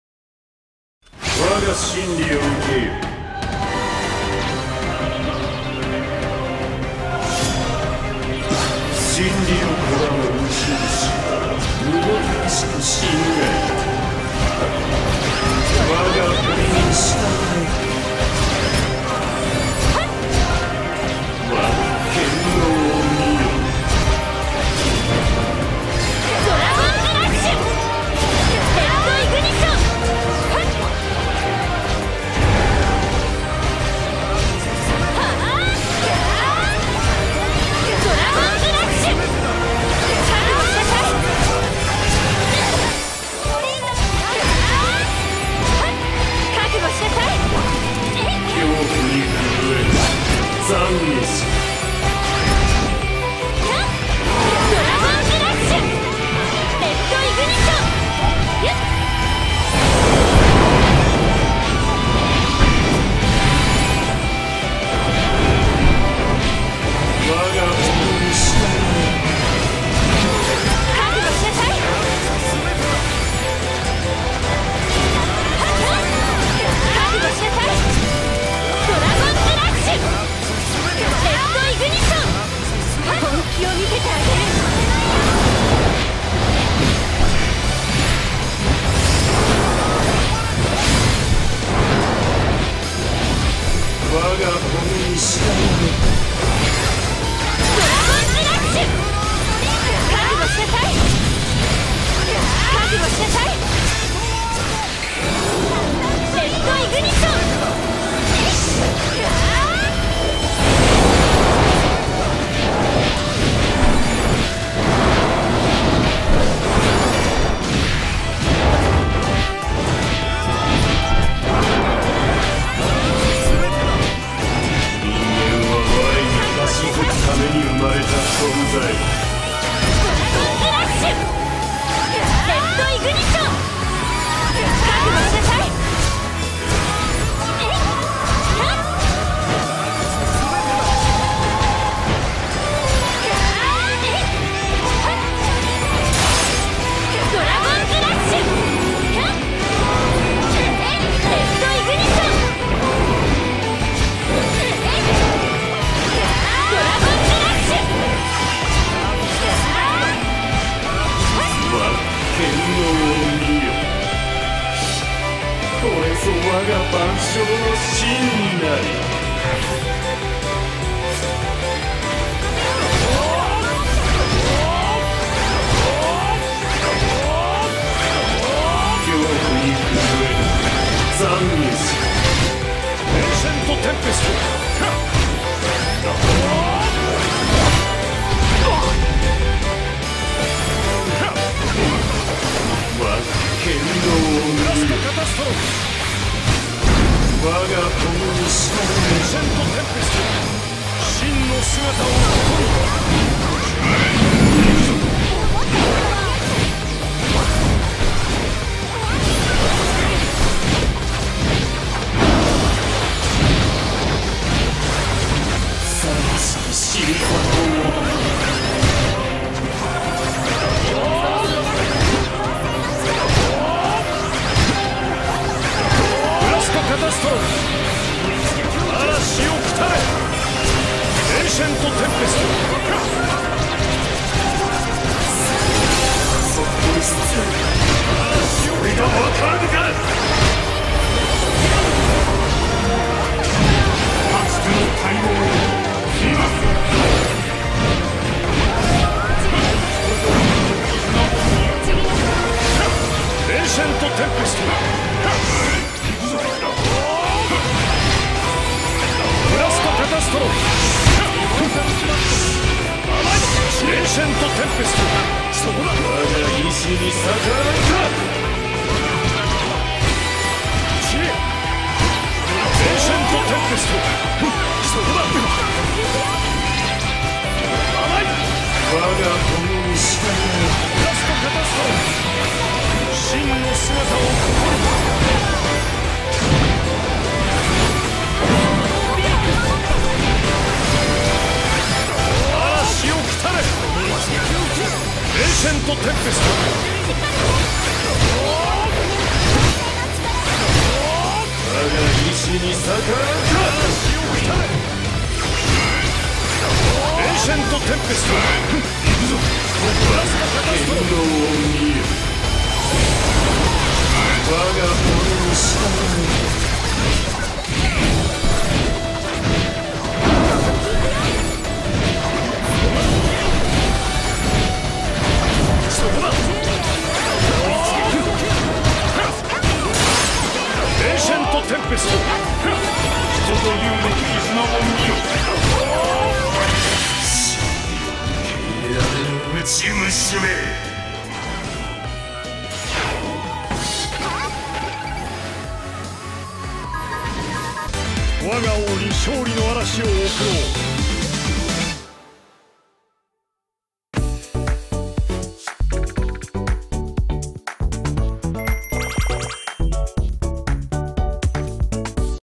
Master Jaldabaoth Coop (Gmym , and Humid POV)
I just realized that it’s possible to dodge the Nihility in p2 but I didn’t shapeshift in this run